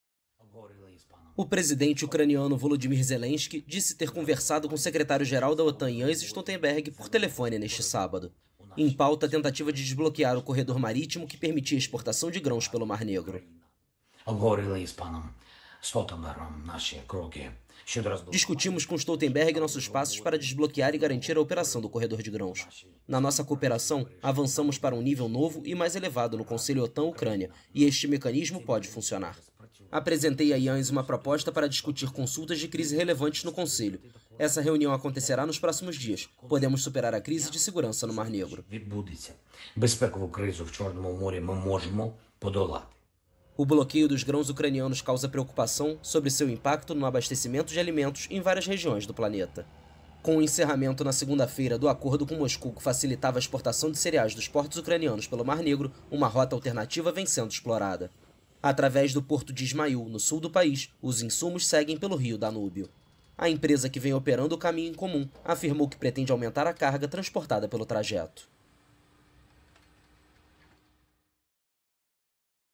Zelensky discute 'desbloqueio' de corredor de grãos com a Otan | AFP
O encerramento, na segunda-feira (17), do acordo que facilitava a exportação de grãos dos portos ucranianos pelo Mar Negro foi discutido neste sábado pelo presidente da Ucrânia, Volodymyr Zelensky, e o secretário-geral da Otan, Jens Stoltenberg.